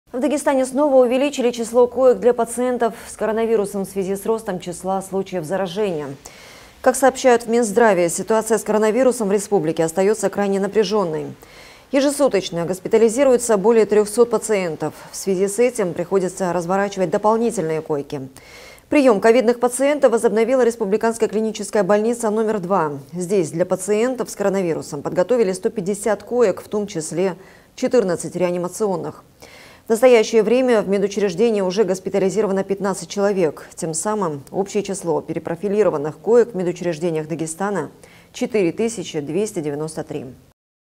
Минздрав РД бьет тревогу
В Дагестане снова увеличили число коек для пациентов с коронавирусом в связи с ростом числа случаев заражения.

Как сообщают в минздраве, ситуация с коронавирусом в республике остается крайне напряженной. Ежесуточно госпитализируется более 300 пациентов. В связи с этим приходится разворачивать дополнительные койки. Прием ковидных пациентов возобновила Республиканская клиническая больница №2. Здесь, для пациентов с коронавирусом подготовили 150 коек, в том числе 14 реанимационных. В настоящее время в медучреждение уже госпитализировано 15 человек. Тем самым общее число перепрофилированных коек в медучреждениях Дагестана 4 тыс. 293.